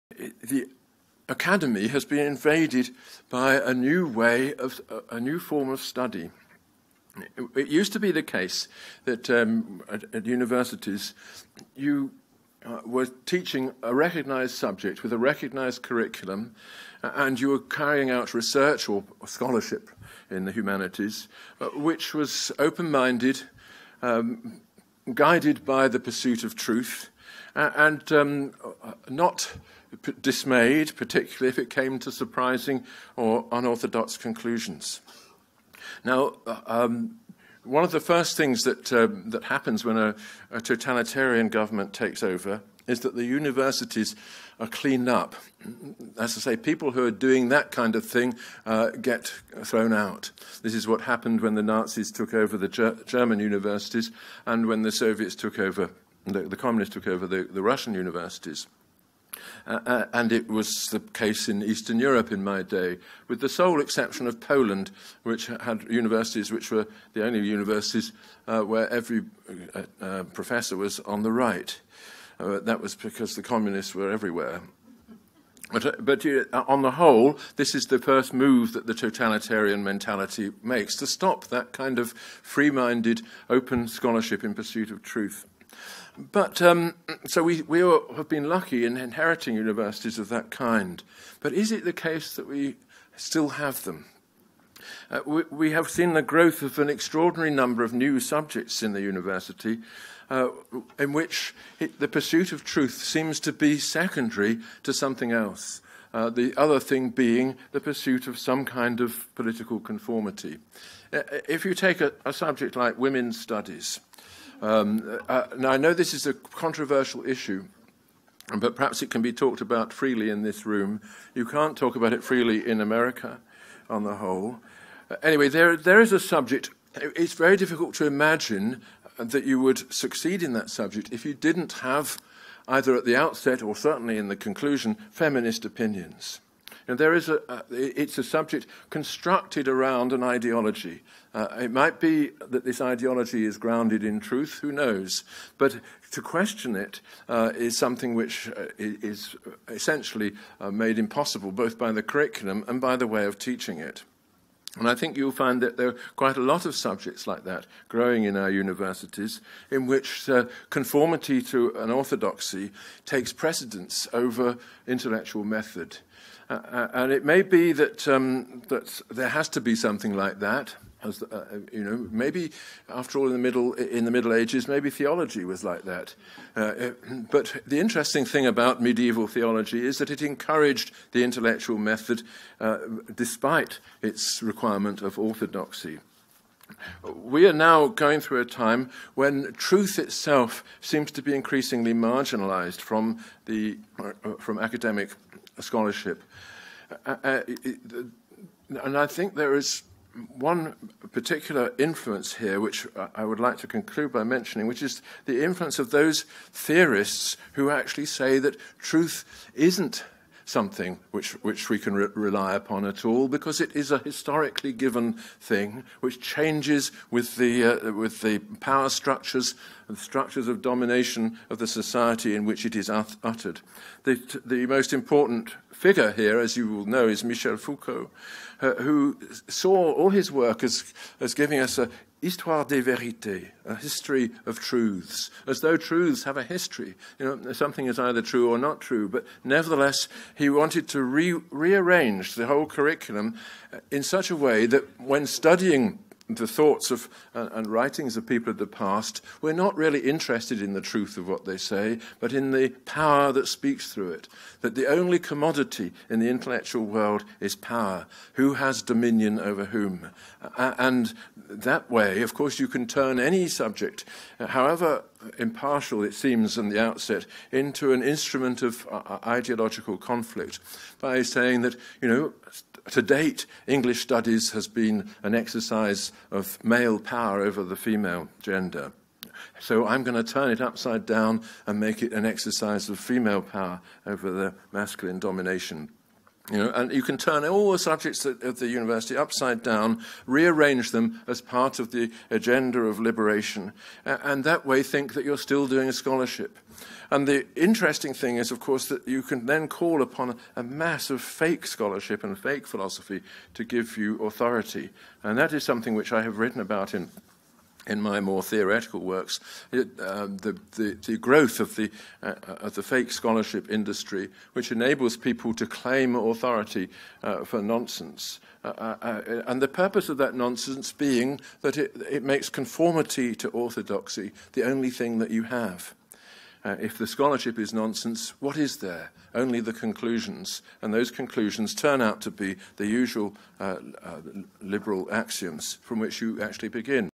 Roger Scruton How Fake Subjects Invaded Academia
Roger Scruton How Fake Subjects Like Women Studies Invaded Academia
[Sir Roger Vernon Scruton:] Source: LYBIO.net
The Academy has been invaded by a new way of – a new form of study. It used to be the case that at universities – you, where teaching a recognized subject with a recognized curriculum and you were carrying out research or scholarship in the humanities, which was open minded guided by the pursuit of truth and not dismayed particularly if it came to surprising or unorthodox conclusions.
Now, one of the first things that that happens when a Totalitarian government takes over is that the universities are cleaned up. That is to say, people who are doing that kind of thing get thrown out. This is what happened when the Nazis took over the German universities and when the Soviets took over – and the Communists took over the Russian universities.
And it was the case in Eastern Europe in my day with the sole exception of Poland, which had universities which were the only universities where every professor was on the right. That was because the Communists were everywhere, but on the whole this is the first move that the Totalitarian mentality makes to stop that kind of free-minded open scholarship in pursuit of truth.
[Roger Scruton:] Source: LYBIO.net
But, so we all have been lucky in inheriting universities of that kind but is it the case that we still have them. We have seen the growth of an extraordinary number of new subjects in the university, in which, the pursuit of truth seems to be secondary to something else. The other thing being the pursuit of some kind of political conformity.
If you take a subject like women’s studies, now I know this is a controversial issue but perhaps it can be talked about freely in this room. You can’t talk about it freely in America on the whole. Anyway there – there is a subject, it’s very difficult to imagine and that you would succeed in that subject if you didn’t have either at the outset or certainly in the conclusion feminist opinions.
And there is a – it’s a subject constructed around an ideology. It might be that this ideology is grounded in truth, who knows, but to question it is something which is essentially made impossible both by the curriculum and by the way of teaching it.
And I think you’ll find that there quite a lot of subjects like that growing in our universities, in which conformity to an orthodoxy takes precedence over intellectual method. And it may be that, that there has to be something like that, you know.
Maybe after all in the middle – in the middle ages maybe theology was like that, but the interesting thing about medieval theology is that it encouraged the intellectual method despite it’s requirement of Orthodoxy.
We are now going through a time when truth itself seems to be increasingly marginalized from the – from academic scholarship.
And I think, there is one particular influence here, which I would like to conclude by mentioning, which is the influence of those theorists who actually say that truth isn’t something, which, which we can rely upon at all because it is a historically given thing, which changes with the – with the power structures and structures of domination of the society in which it is uttered.
The most important figure here as you all know is Michel Foucault, who saw all his work as giving “une histoire de vérité” a history of truths. As though truths have a history, you know, something is either true or not true but nevertheless he wanted to rearrange the whole curriculum in such a way that when studying the thoughts of and writings of people of the past, we’re not really interested in the truth of what they say but in the power that speaks through it.
That the only commodity, in the intellectual world is power, who has dominion over whom. And that way, of course you can turn any subject however impartial it seems and the outset into an instrument of ideological conflict by saying that you know, to date English studies has been an exercise of male power over the female gender.
So I’m going to turn it upside down and make it an exercise of female power over the masculine domination. You know and you can turn all the subjects at the university upside down rearrange them as part of the agenda of liberation and that way think that you’re still doing a scholarship. And the interesting thing is of course that you can then call upon a massive fake scholarship and a fake philosophy to give you authority.
And that is something, which I have written about in my more theoretical works. The, the growth of the at the fake scholarship industry, which enables people to claim authority for non-sense and the purpose of that non-sense being that it makes conformity to orthodoxy the only thing that you have.
If the scholarship is non-sense what is there, only the conclusions and those conclusions turn out to be the usual liberal axioms from which you actually begin.